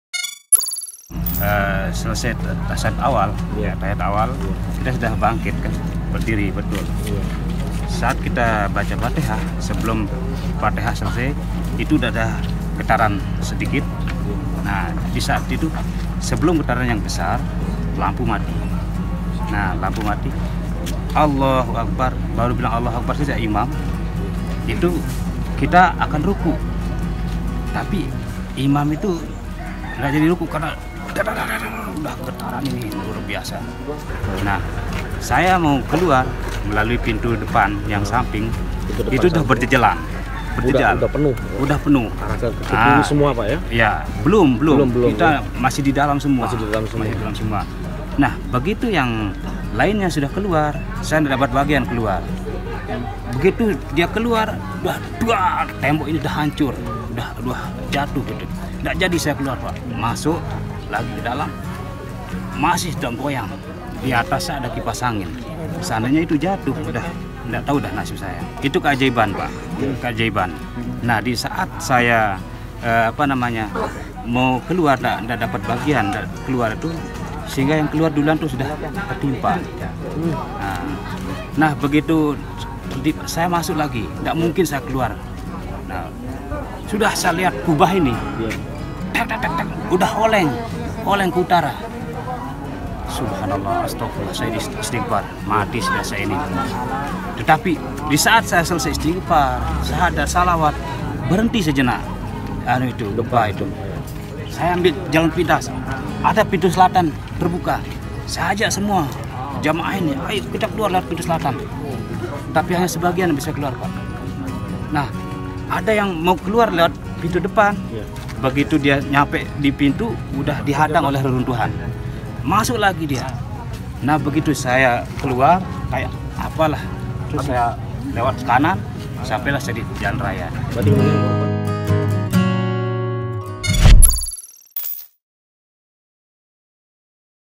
Kisah Perjuangan Warga Lombok Selamatkan Diri dari Reruntuhan
Seorang warga Lombok bernama Syarafuddin saat itu tengah salat berjemaah bersama warga lainnya. Seketika gempa menerjang, membuat bangunan masjid roboh. Dia pun menceritakan perjuangannya hingga berhasil selamat dan keluar dari reruntuhan.